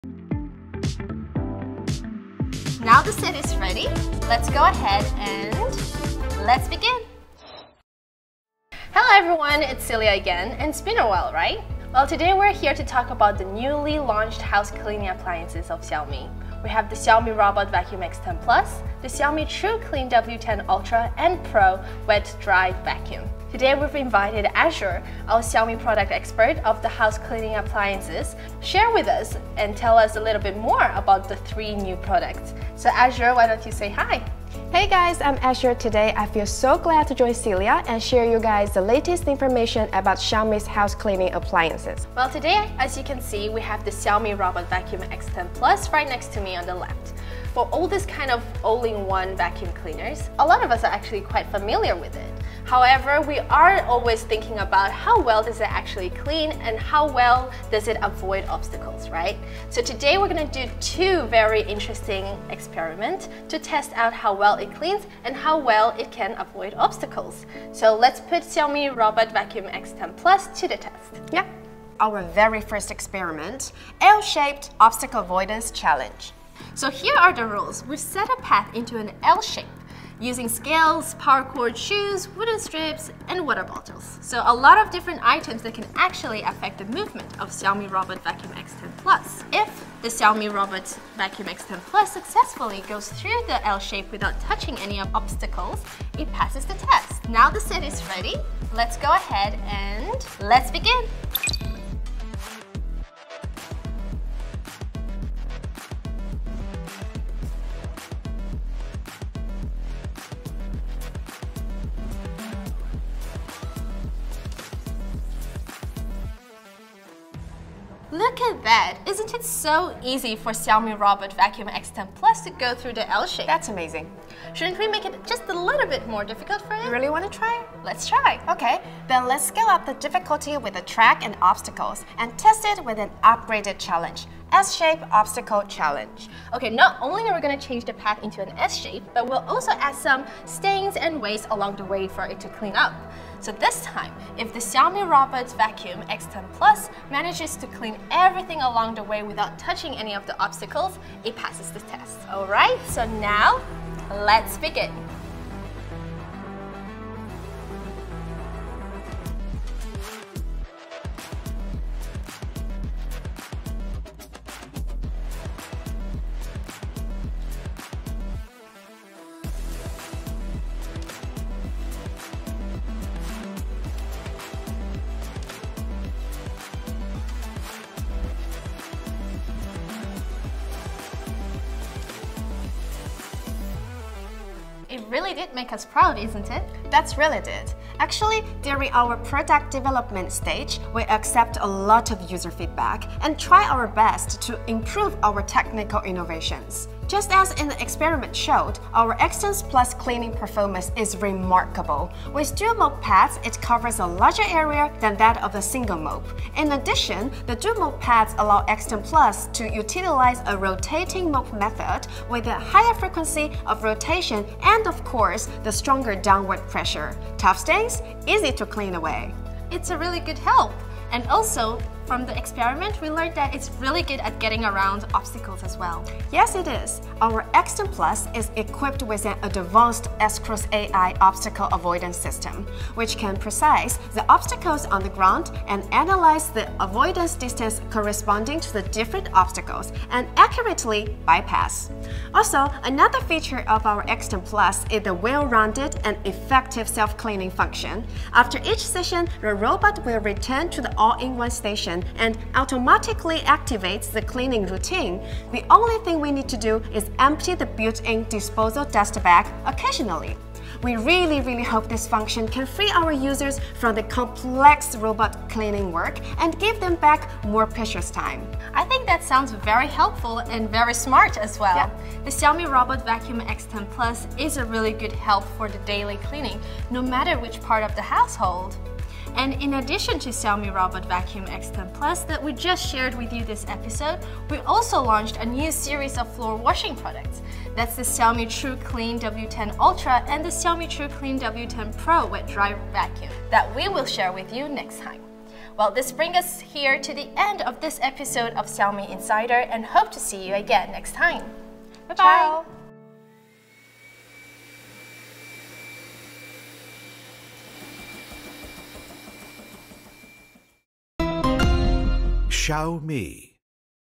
How well can smart robot vacuums avoid obstacles? | Xiaomi Insider
Watch this XiaomiInsider episode to learn about XiaomiRobotVacuum X10+'s obstacle avoidance ability.